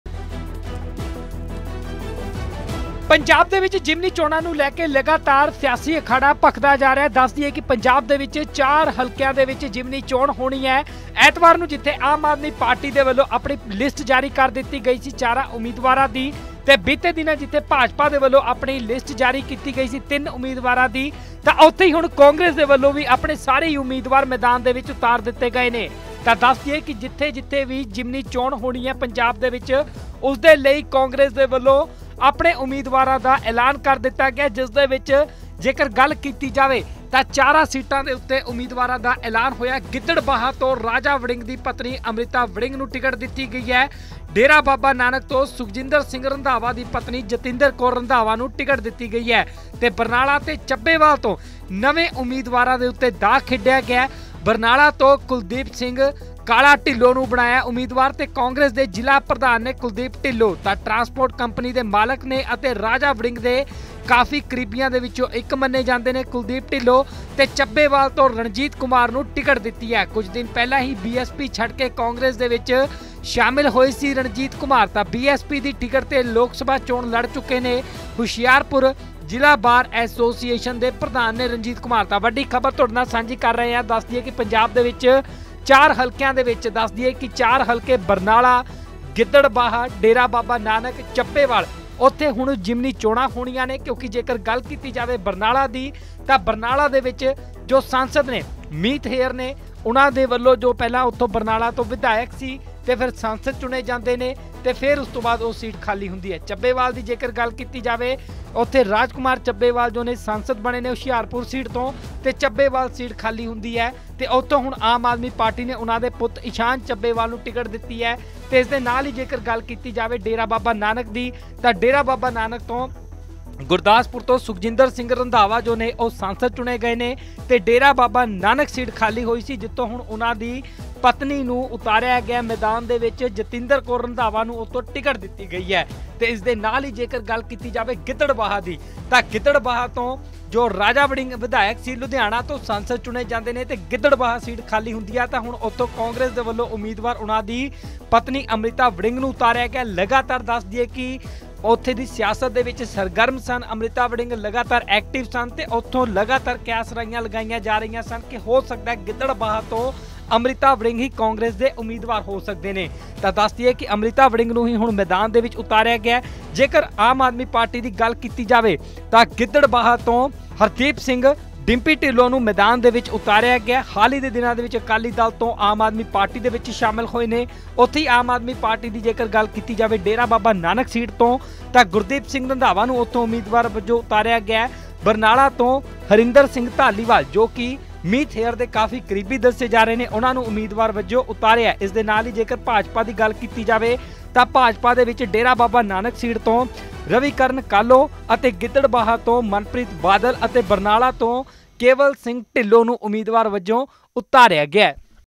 Gurdaspur News | ਕਾਂਗਰਸ ਨੇ ਜ਼ਿਮਨੀ ਚੋਣ ਲਈ ਐਲਾਨੇ ਉਮੀਦਵਾਰ
ਪੰਜਾਬ ਦੇ ਵਿੱਚ ਚਾਰ ਹਲਕਿਆਂ ਚ ਹੋਣ ਵਾਲੀ ਜਿਮਨੀ ਚੋਣ ਨੂੰ ਲੈ ਕੇ ਕਾਂਗਰਸ ਦੇ ਵੱਲੋਂ ਵੀ ਆਪਣੇ ਉਮੀਦਵਾਰਾਂ ਦਾ ਐਲਾਨ ਕਰ ਦਿੱਤਾ ਹੈ। ਗਿੱਦੜਬਾਹਾ ਤੋਂ ਰਾਜਾ ਵੜਿੰਗ ਦੀ ਪਤਨੀ ਅੰਮ੍ਰਿਤਾ ਵੜਿੰਗ ਦੇ ਉੱਤੇ ਕਾਂਗਰਸ ਦੇ ਵੱਲੋਂ ਵੱਡਾ ਦਾਅ ਖੇਡਿਆ ਗਿਆ ਹੈ।